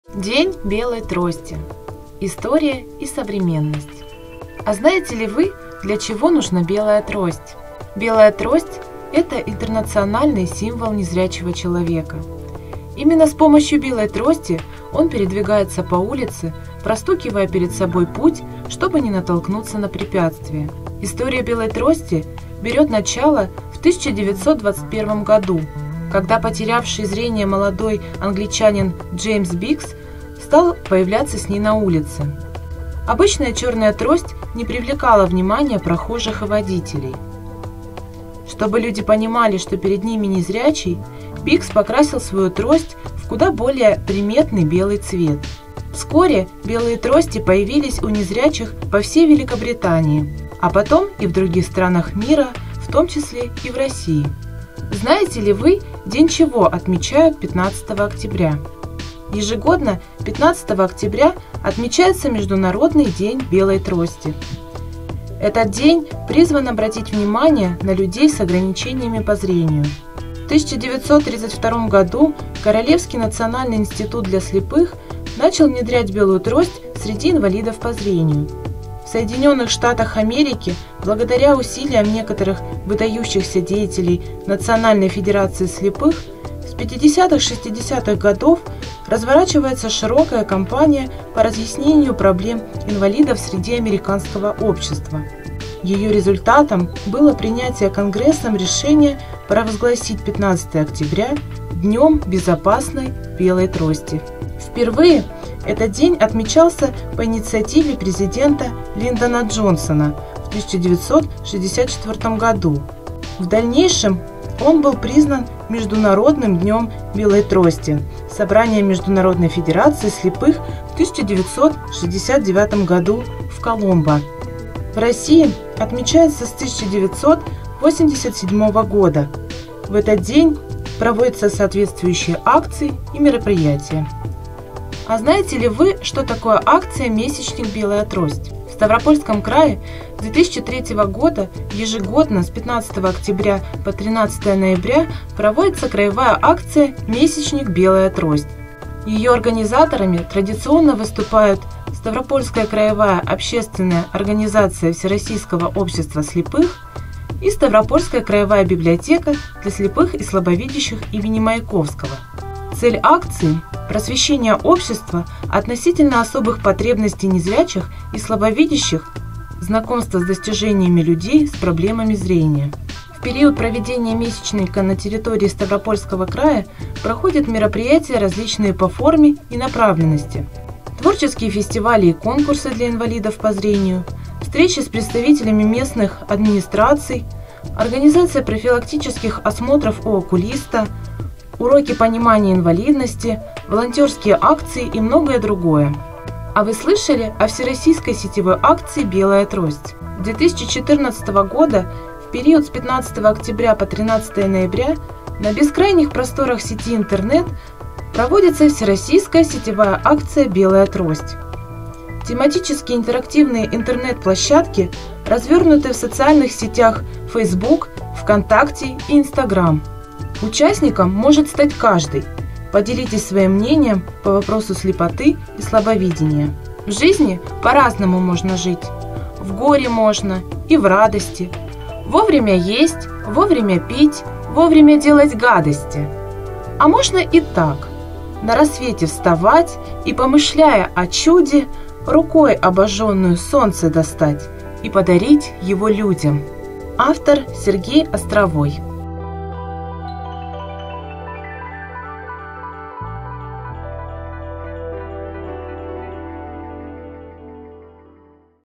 День белой трости. История и современность
Международное сообщество обозначило на календарях немало знаменательных и памятных дат. В их числе – Международный день Белой трости (International White Cane Safety Day),отмечаемый 15 октября. Но это – не праздник. Это своеобразный «знак беды», напоминающий обществу о существовании рядом людей с ограниченными физическими возможностями, о помощи и о солидарности.
Трость для слепого – это не просто инструмент и опознавательный знак для условно здоровых, но ещё и его «глаза». В специально разработанной методике по ориентированию и мобильности, предназначенной для незрячих и слабовидящих людей, среди основных функций далеко не на последнем месте стоит «сигнальная» функция. Помимо этого, особо стоит отметить следующие аспекты применения трости: «Защитный», «Опорный», «Генератор звука», «Ощупывательный» и др. Звук от удара тростью о тротуар или мостовую позволяет незрячему услышать окружающее пространство и ощутить «высокие» препятствия. Например, дома, деревья, столбы, припаркованные машины. Скольжение трости по поверхности дороги – определить наличие «низких» препятствий: бордюров, канализационных люков, выбоин, ступенек. И, кроме того, почувствовать изменение покрытия: асфальт-гравий, плитка-бетон, брусчатка-земля и т.д.
Как средство определения пути, трость известна уже давно, но история Белой трости как символа слепоты началась только с 1921 года.